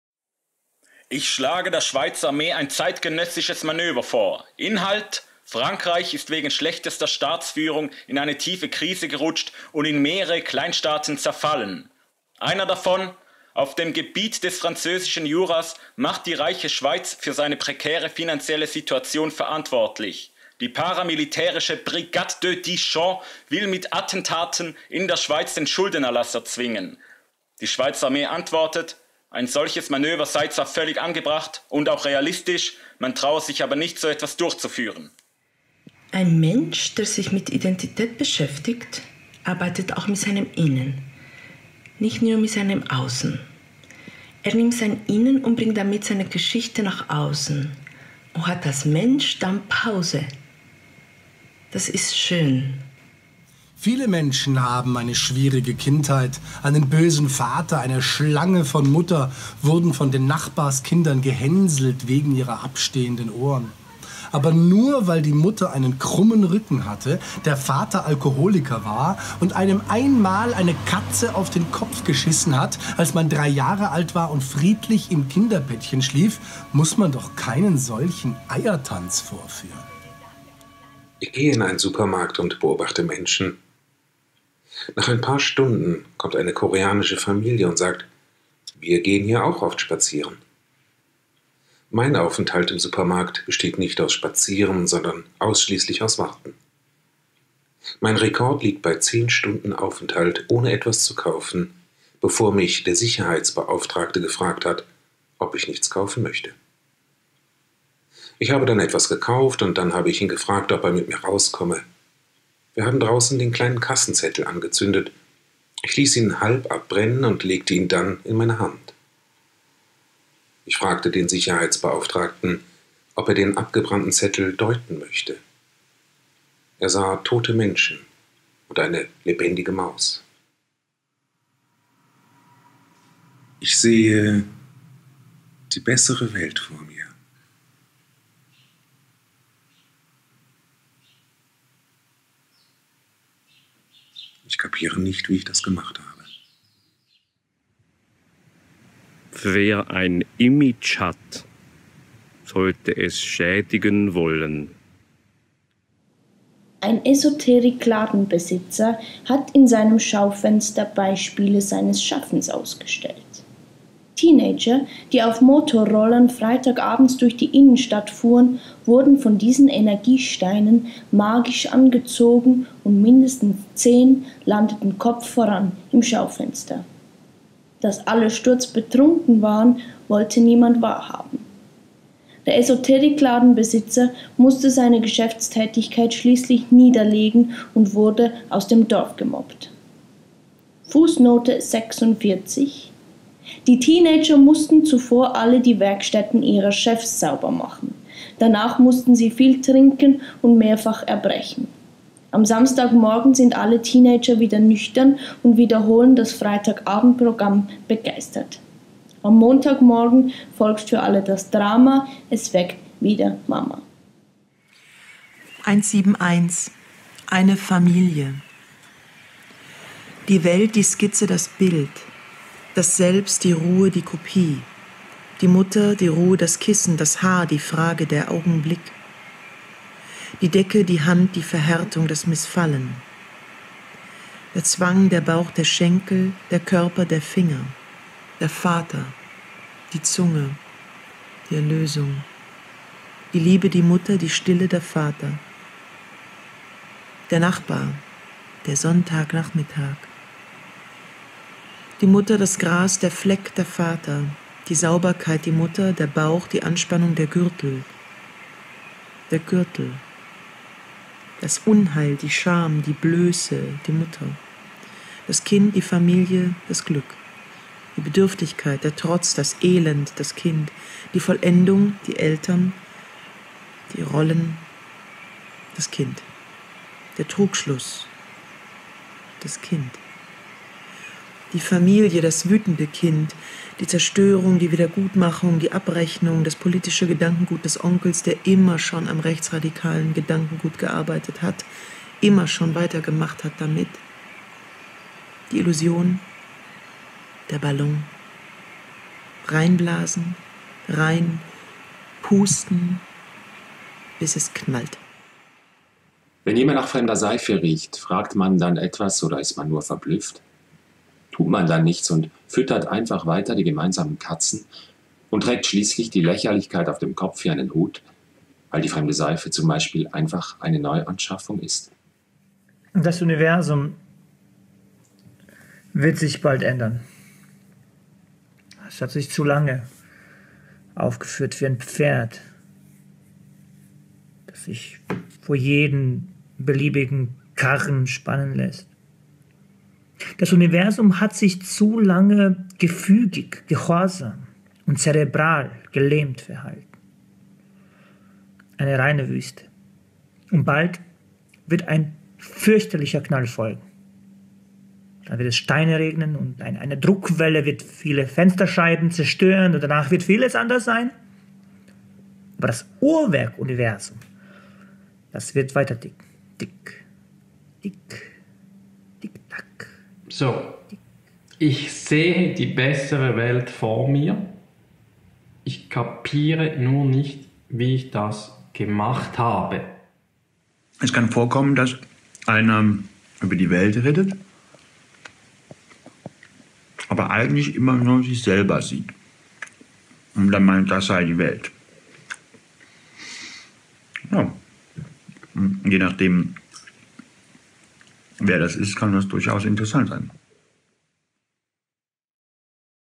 Michael Stauffer: Jeden Tag das Universum begrüßen
Michael Stauffer grüßt das Universum! In seinem bei Voland & Quist erschienenen Buch macht er das in 496 scharfsinnigen wie komischen Beobachtungen, Fragen und Erkenntnissen, in diesem Video geben er und namhafte Kollegen und Kolleginnen einen Einblick. Es lesen und grüßen:
Marko Stefanovic
Diana Rojas
Jess Jochimsen
Marcel Beyer
Martin Rapold
Michelle Steinbeck
Nora Gomringer
Peter Stamm
Saša Stanišić
Michael Stauffer
Urs Jucker

Michael Stauffer
Jeden Tag das Universum begrüßen
368 Seiten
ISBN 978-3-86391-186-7
Euro 22,00 (D)